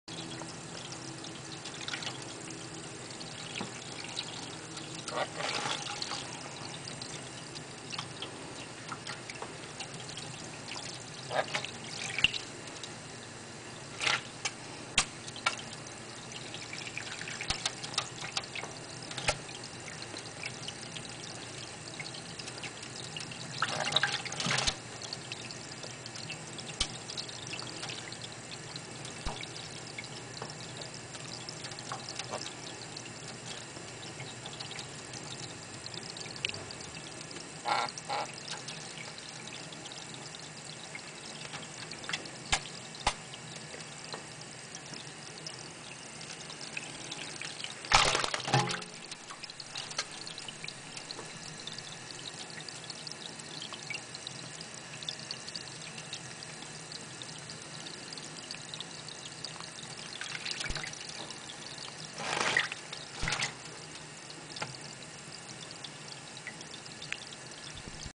水浴び
コガネの水浴び．．．
My bird taking a bath. . .